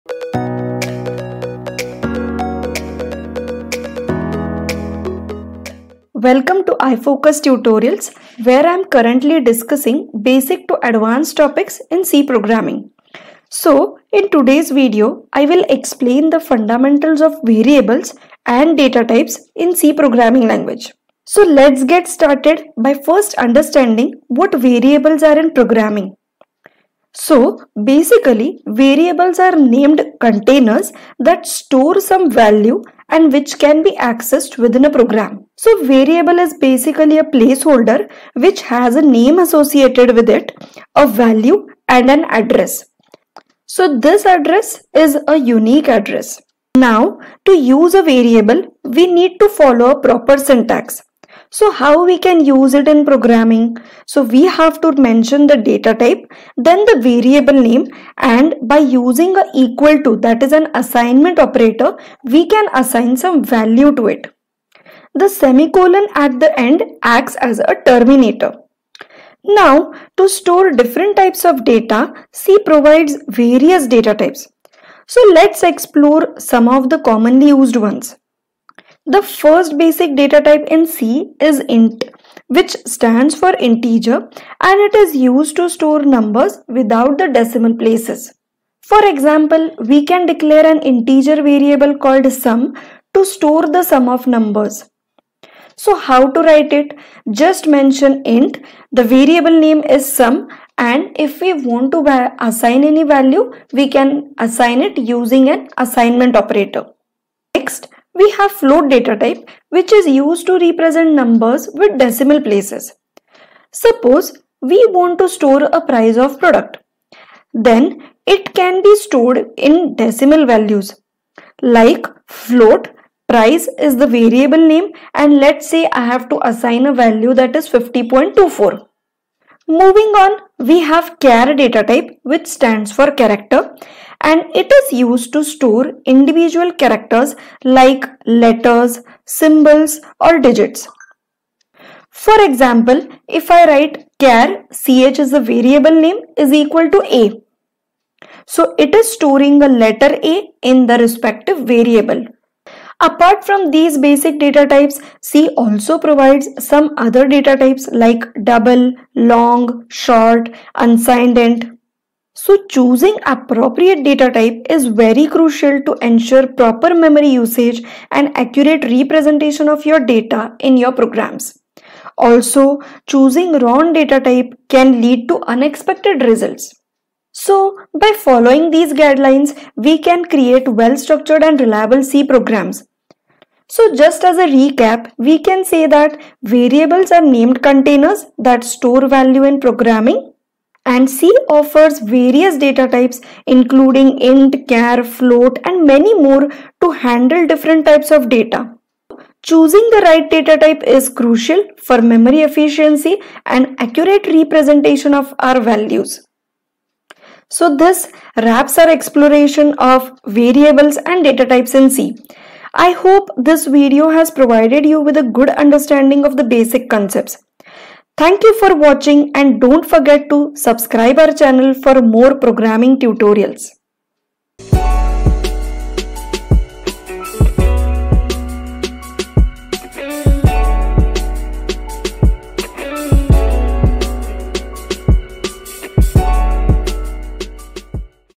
Master Variables & Data Types in Programming: Essential C Concepts Explained!
"🚀 Master Variables & Data Types in Programming | C Programming Fundamentals Explained! 🚀

In this engaging and informative video, we dive deep into the essential concepts of variables and data types in programming. Whether you're a beginner or looking to strengthen your programming skills, understanding these fundamental building blocks is crucial for success.

🔎 What you'll learn:

- The significance of variables and how they act as containers for data.
- Explore different data types, such as 'int', 'float', 'char', and more, and when to use them effectively.
- Discover the role of data types in memory management and optimizing your code.
- Step-by-step explanations and practical code examples for a better grasp of the concepts.
- Insights into common mistakes and best practices to write robust and efficient code.

💡 Who should watch:

- Beginners eager to start their programming journey.
- Students looking to strengthen their C programming knowledge.
- Developers seeking a refresher on the fundamentals.
- Programming enthusiasts hungry for knowledge!